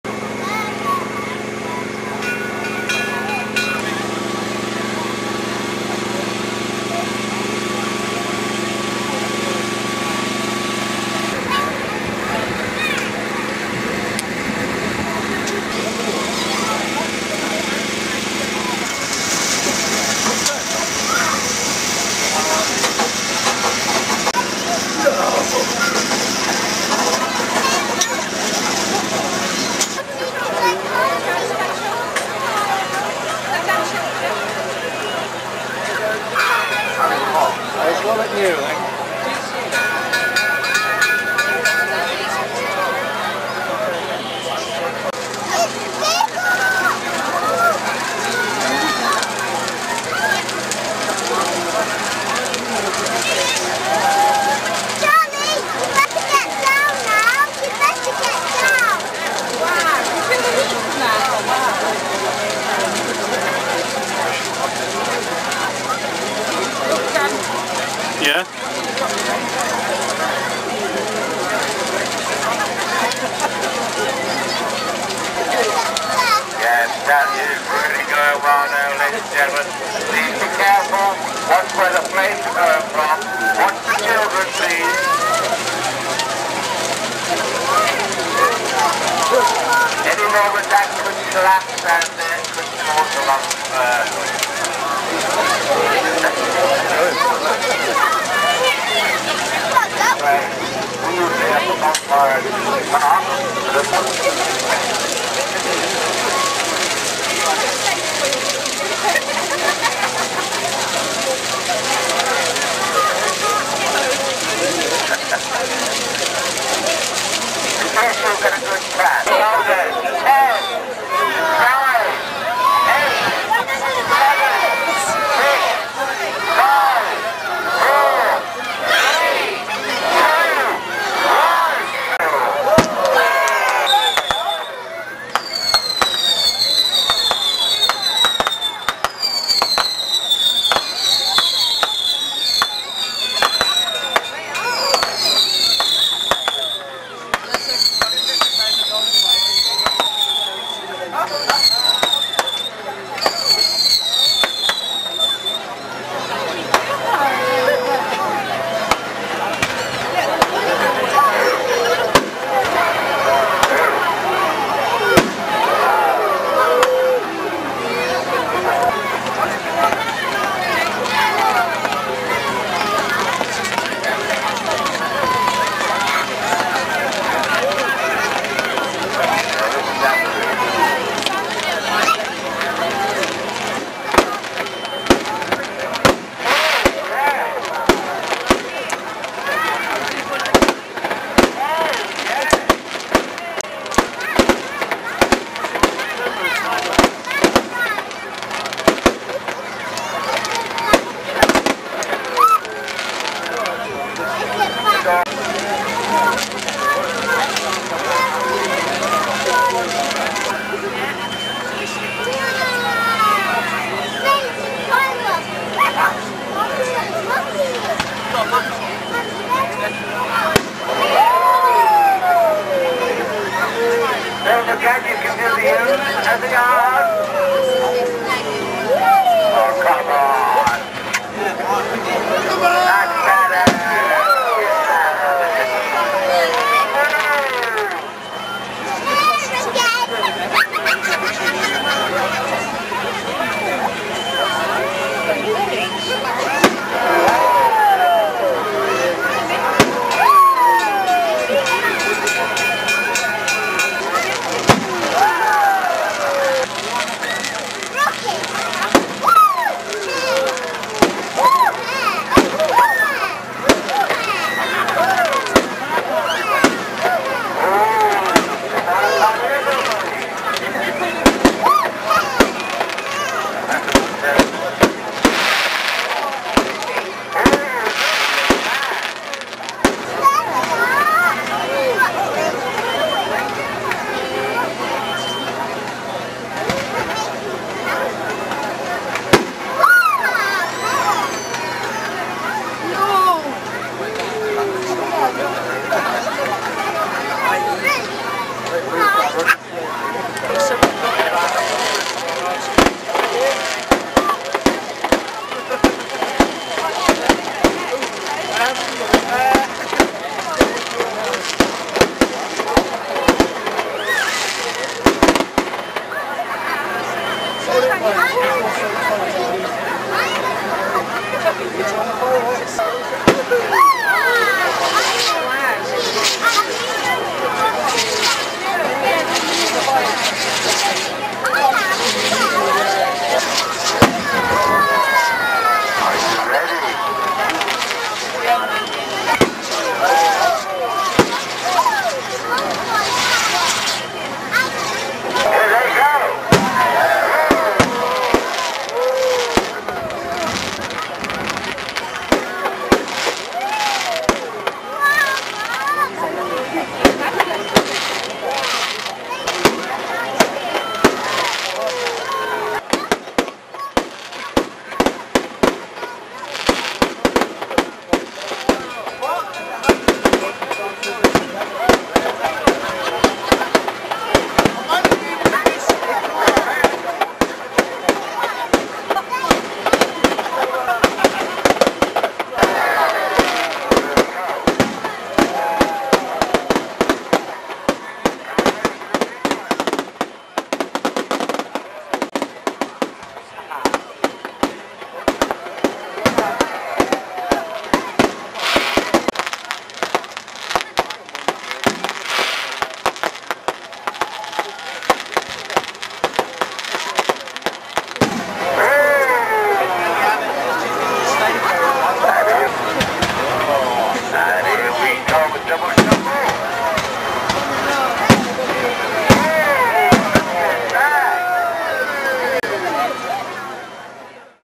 Rotary Club Bonfire & Firework Display at Tewkesbury Abbey 7/11/09
Quite an impressive display considering it was free! A donation was made.